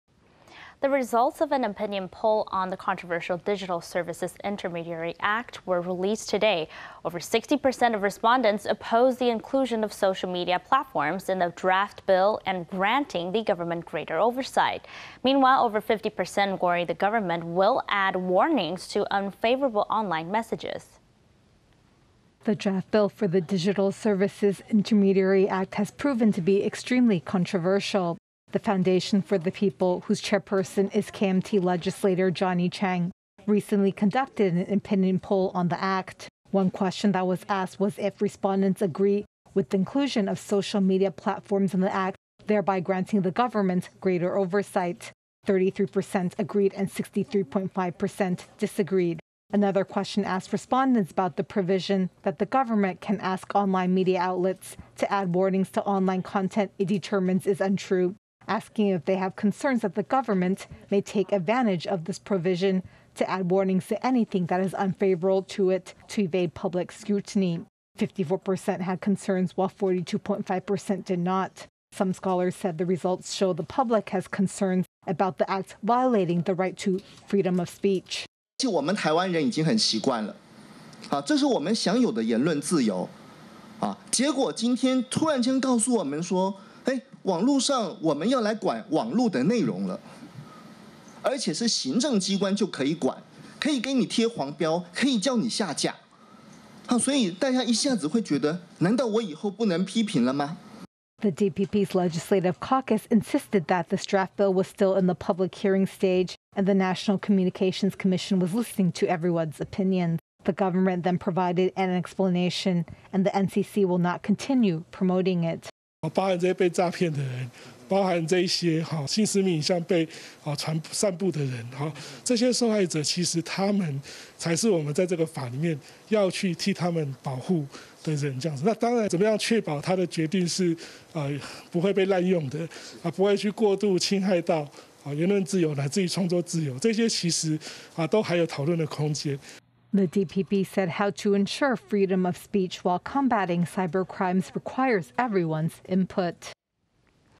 Poll Shows Opposition to Controversial Digital Law｜20220831 PTS English News公視英語新聞
The results of an opinion poll on the controversial Digital Services Intermediary Act were released on Aug. 31. Over 60 percent of respondents opposed the inclusion of social media platforms in the draft bill and granting the government greater oversight. Meanwhile, over 50 percent worry the government will add warnings to unfavorable online messages.

今天有一項關於數位中介服務法的民調發布，有超過六成的受訪民眾反對草案將網路社交平台納入，強化政府監管，有過半的受訪者擔憂政府會將不利的網路訊息都加上警語。有學者認為這反映出民意擔憂數位中介服務會限制網路言論自由。